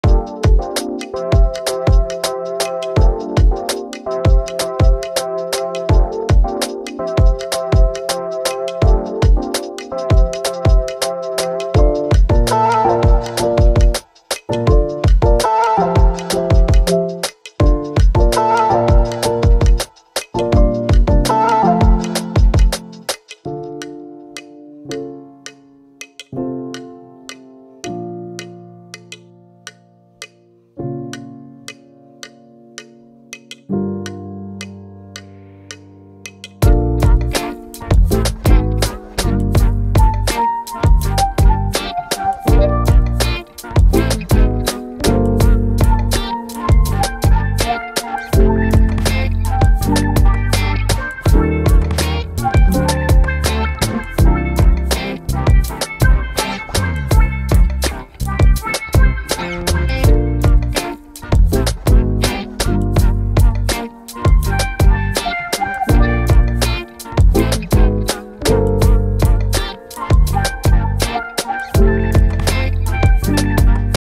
Unable to move icons on screen in Tecno smartphone
When sorting icons on desktops, the problem may arise that they cannot be moved at all.
The problem is most likely not related to bugs in the operating system and can be solved by changing some settings.